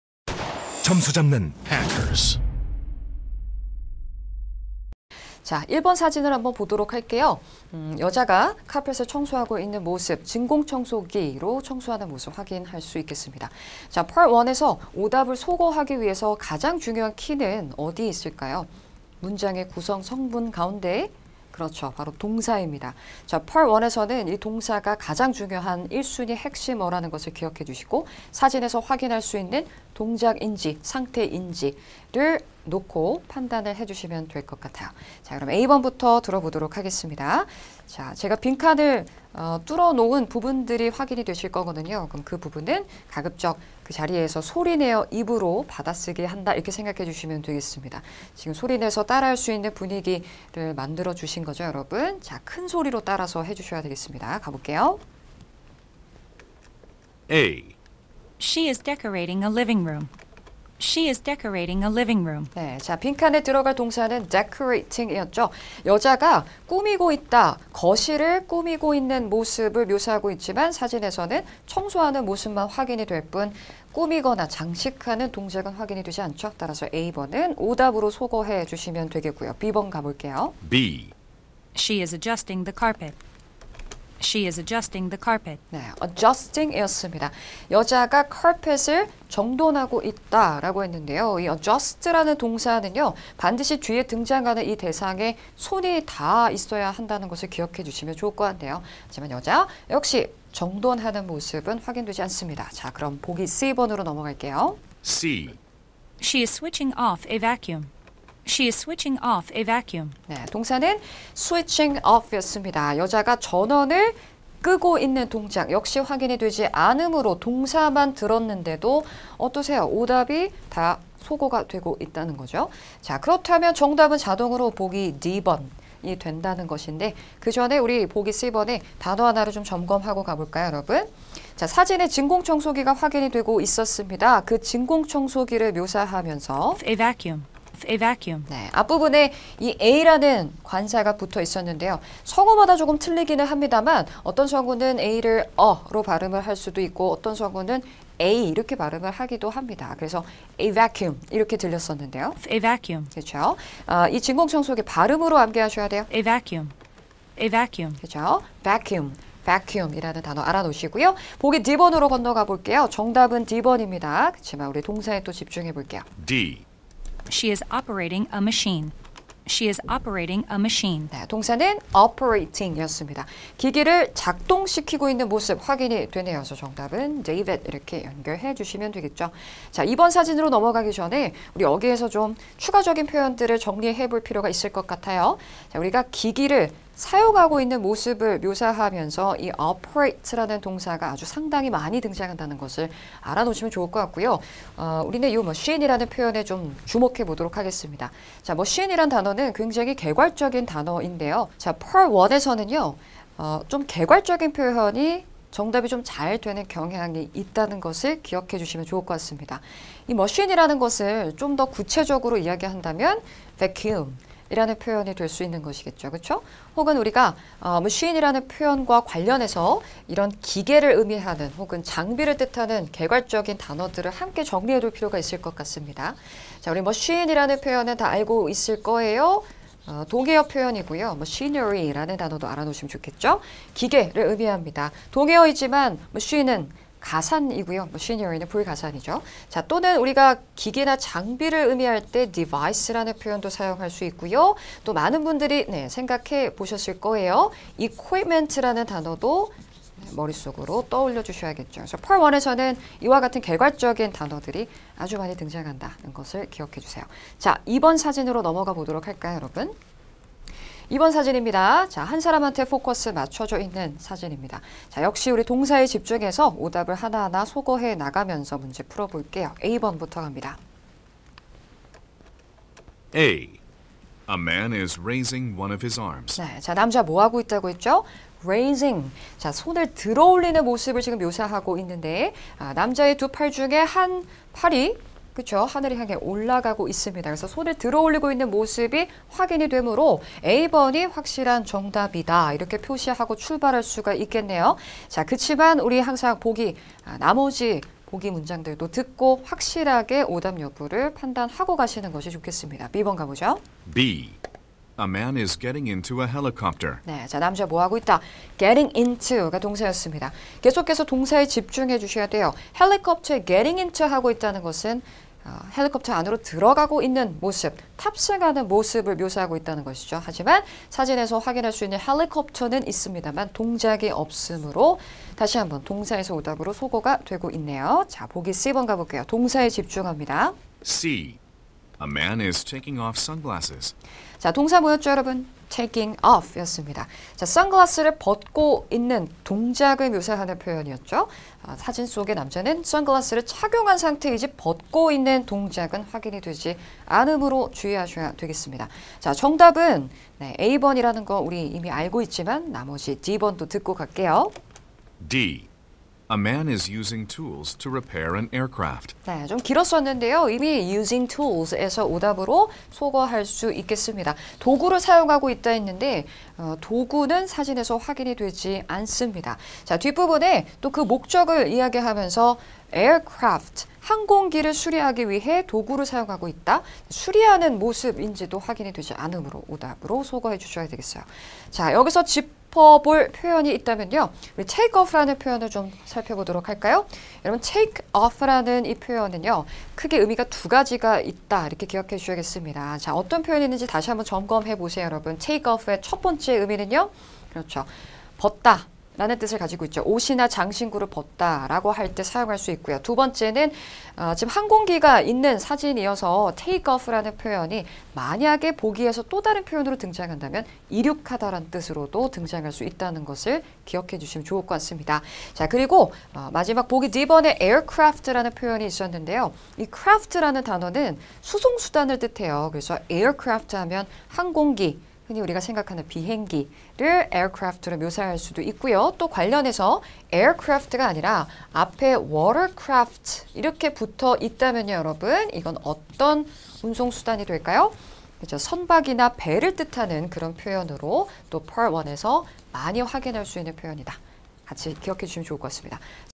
은근히 헷갈리는 토익LC 파트1 공략법! [토익공부방법/해커스토익]
방대한 토익단어, 빠르게 공부하는 법은 없을까?!
신영화쌤이 알려주는 토익단어 공부방법